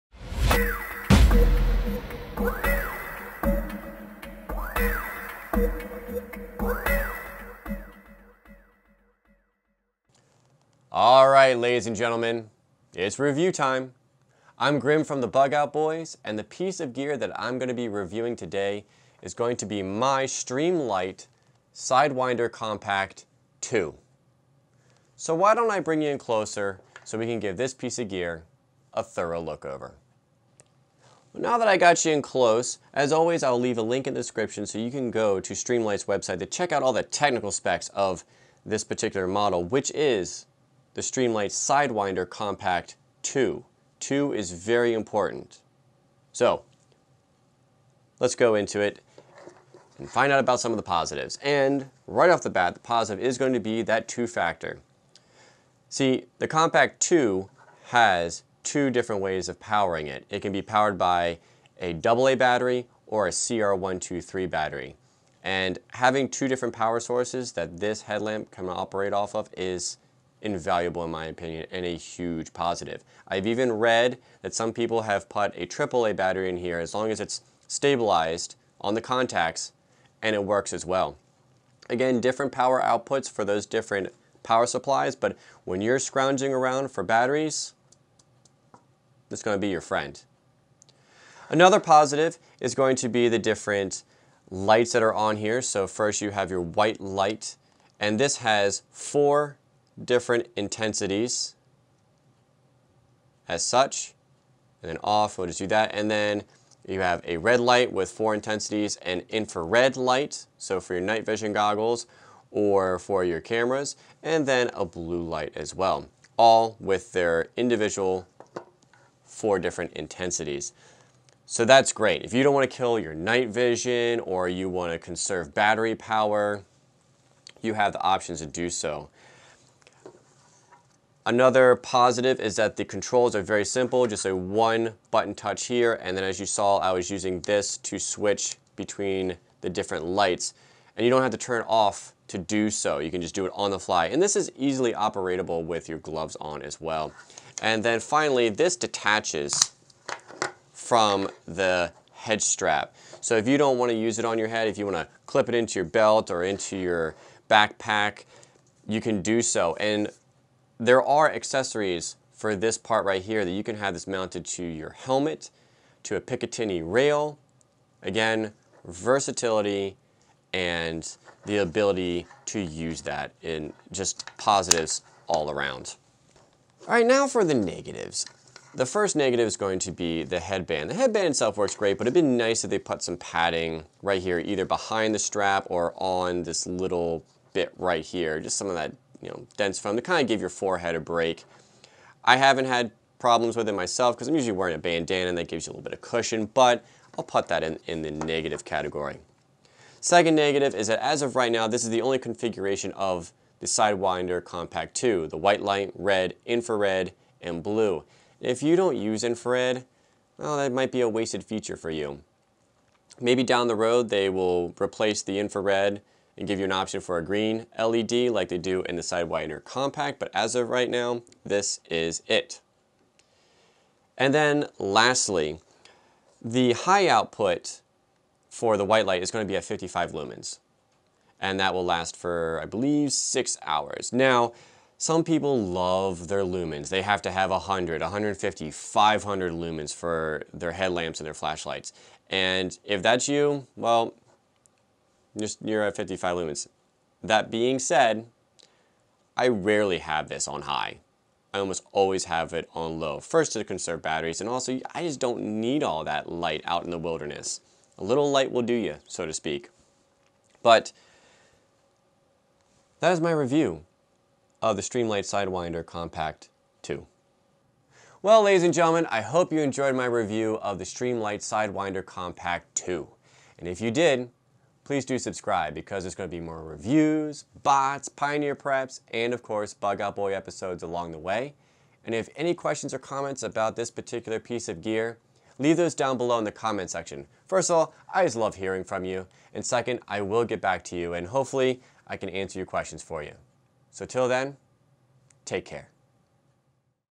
Sidewinder Compact II Review
Grimm goes over the Sidewinder Compact II